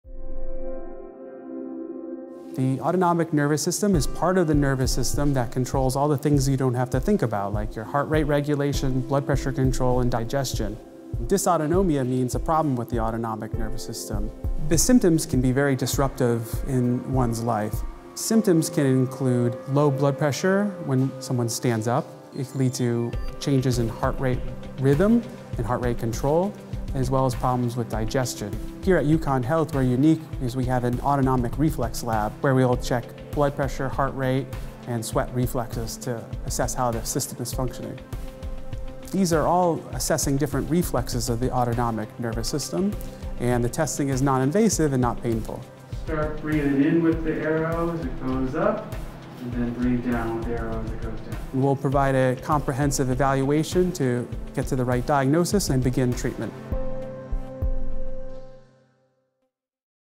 UConn Health Minute: Diagnosing Dysautonomia
Your autonomic nervous system controls your heart rate, blood pressure, digestion so when there is a problem – called dysautonomia – it can wreak havoc in your life. UConn Health is unique because it has an autonomic reflex lab that tests specifically for dysautonomia. The painless, non-invasive tests help lead to a prompt, accurate diagnosis and treatment plan.